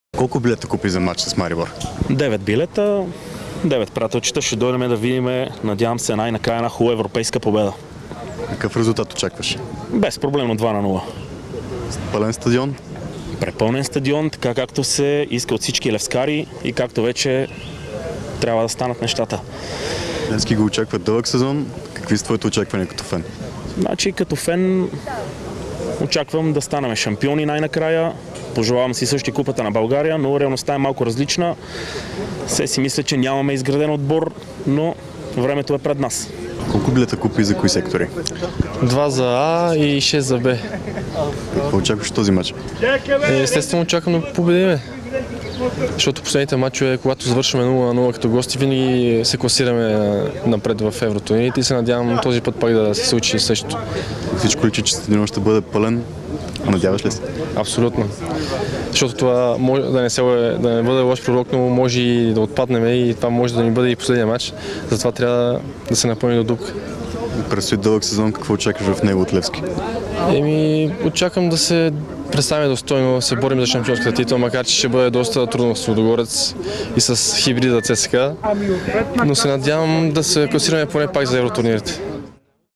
Феновете на Левски са оптимисти преди реванша с Марибор
Феновете на Левски очакват победа от любимците си срещу Марибор.